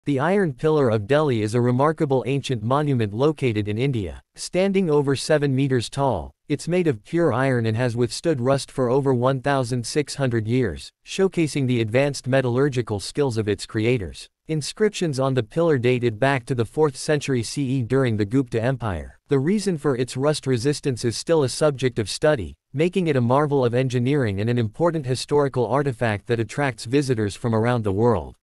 The Iron Pillar Of Delhi,India
This is the video that tells about iron pillar in Delhi,India.It is famous for the rust-resistant composition of the metals used in its construction